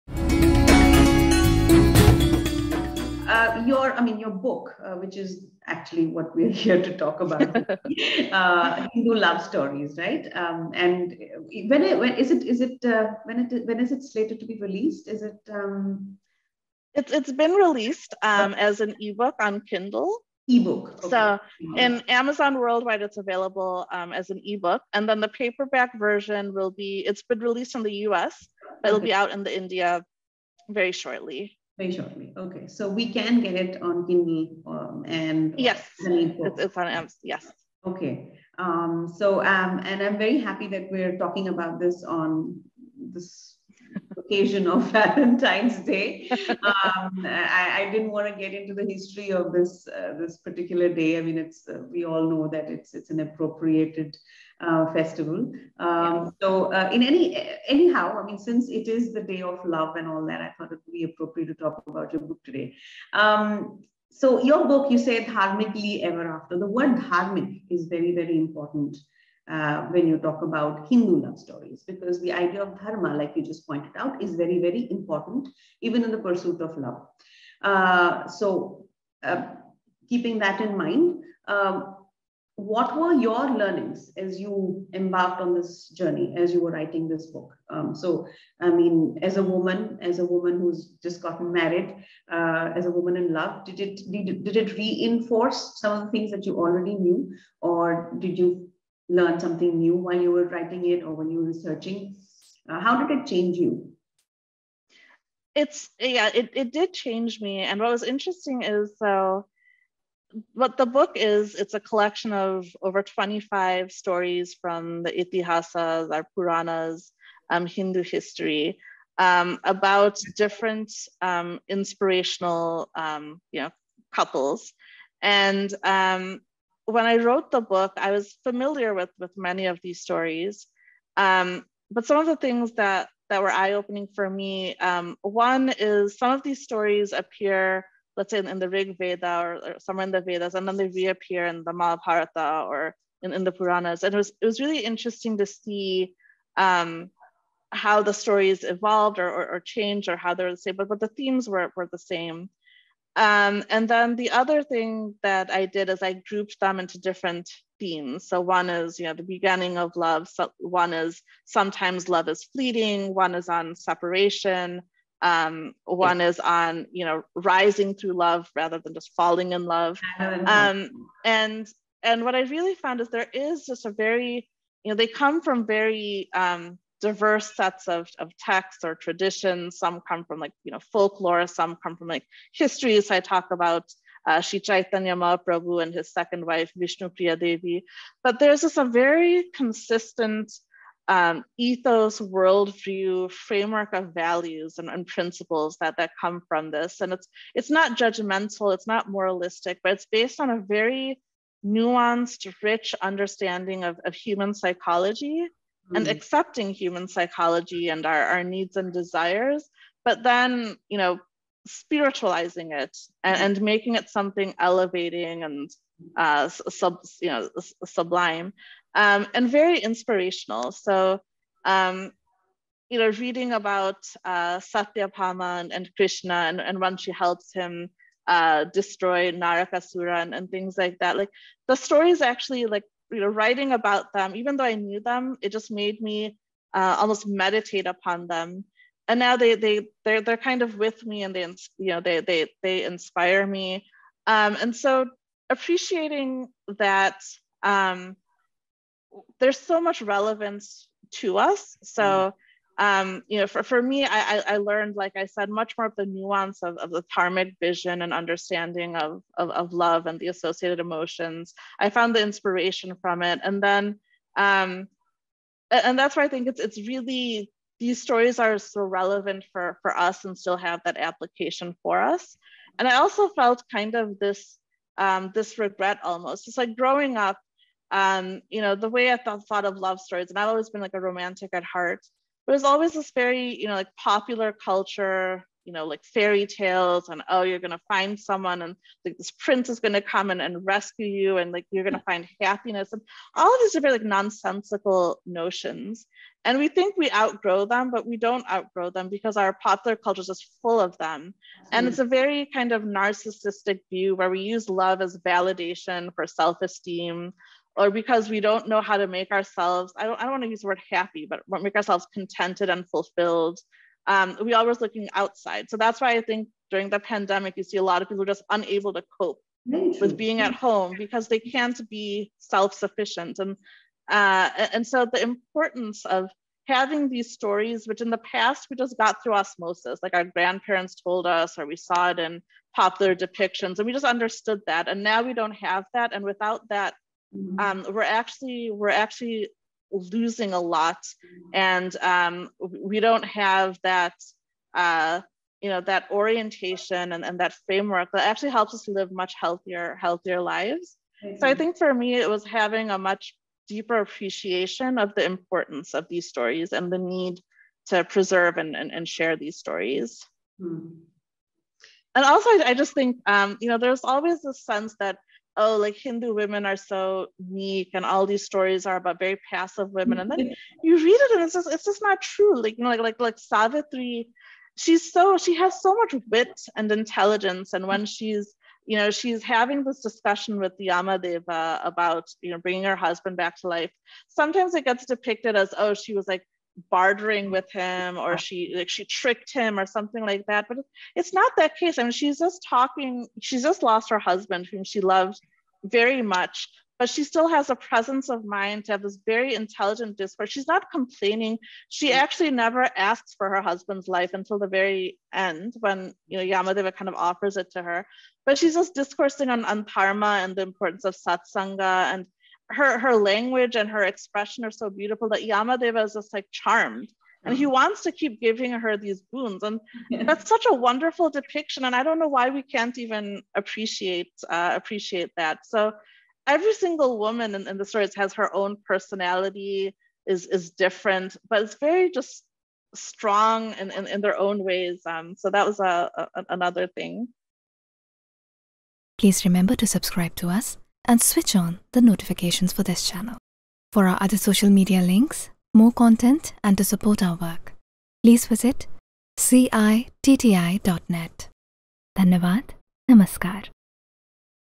Hindu Love Stories: attorney & novelist Aditi Banerjee Malakar explains her journey of discovery
Attorney & novelist Aditi Banerjee Malakar explains her journey of discovery when writing her new book, Hindu Love Stories, inspired from Dharmic ithiasa & wisdom